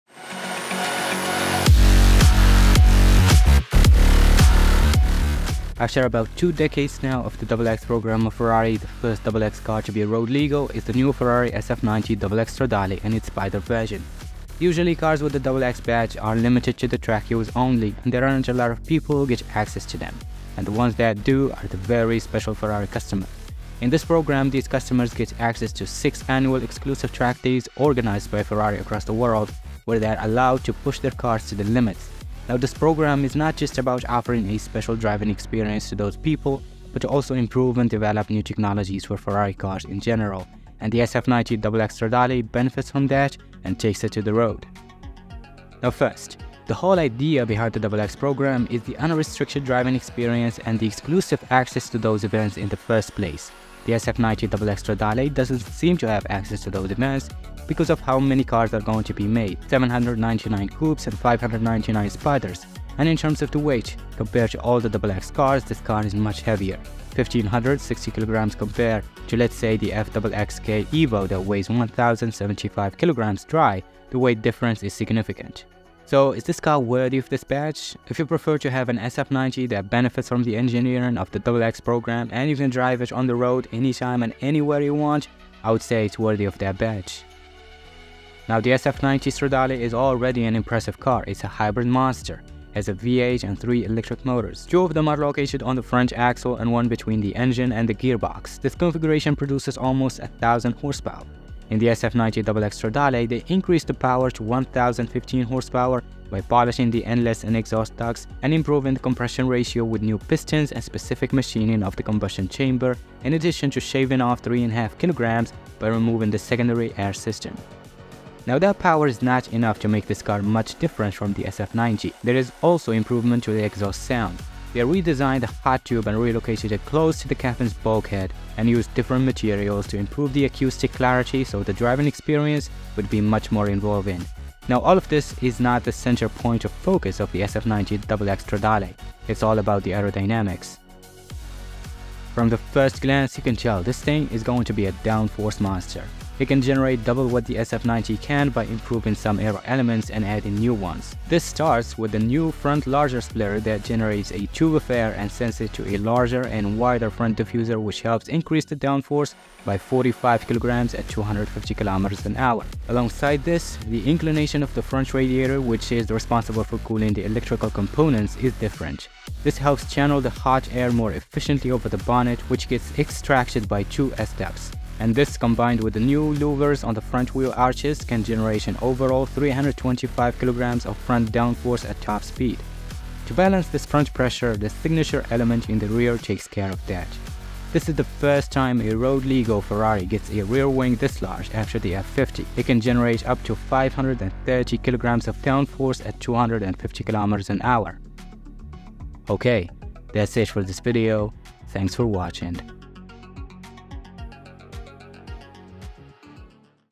The First Ferrari To Escape The XX Program
After about two decades now of the xx program of ferrari, the first xx car to be road legal is the new ferrari SF90 XX Stradale and its spyder version. Usually cars with the XX badge are limited to the track use only and there aren’t a lot of people who get access to them and the ones that do are the very special ferrari customers. In this program, these customers get access to six annual exclusive track days organized by Ferrari across the world. Where they are allowed to push their cars to the limits. Now this program is not just about offering a special driving experience to those people but to also improve and develop new technologies for ferrari cars in general and the SF90 XX Stradale benefits from that and takes it to the road.